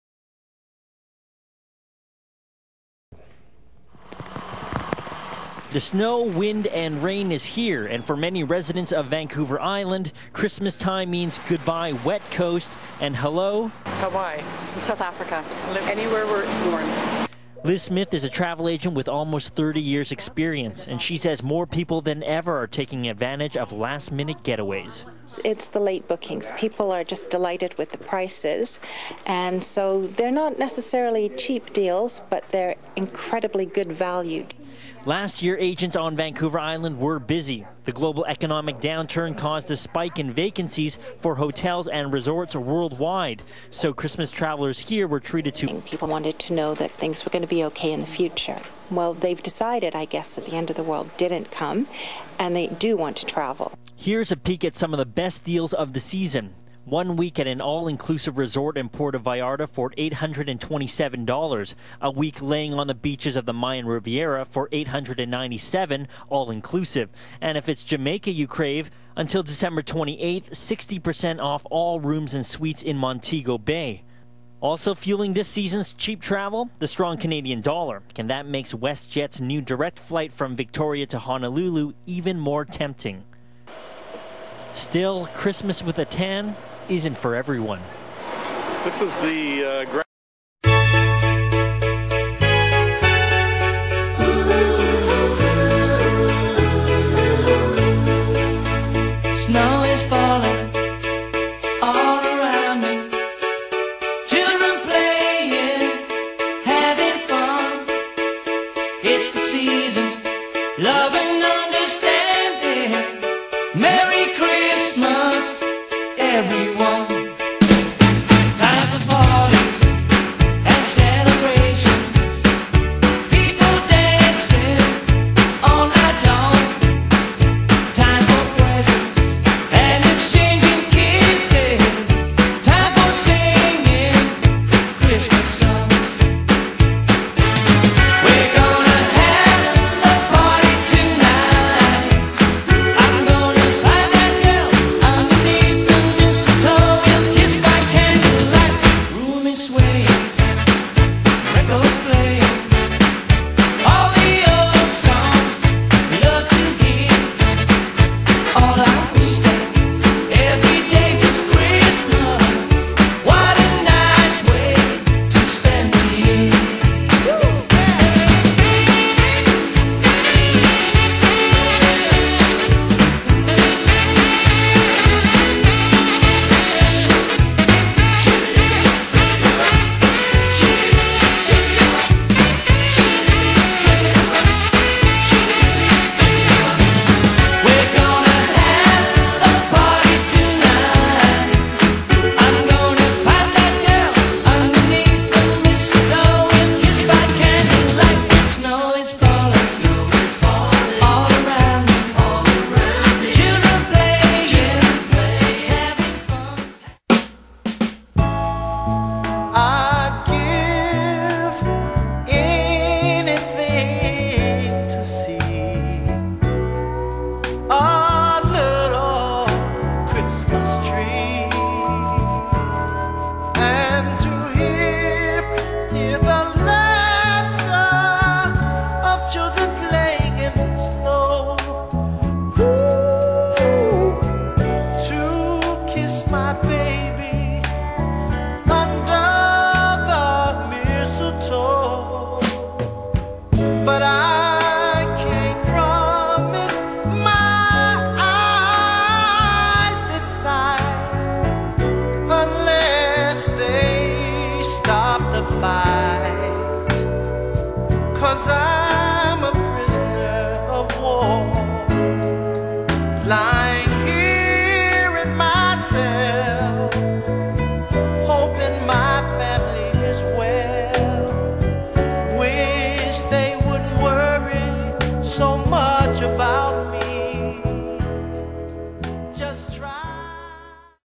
Athlone Travel featured on CHEK TV Dec 15, 2009
Athlone Travel - Canada's Top Travel Agency on TV Dec 15, 2009. The world has not ended and travellers flock to purchase the special deals. WestJet Hawaii Honolulu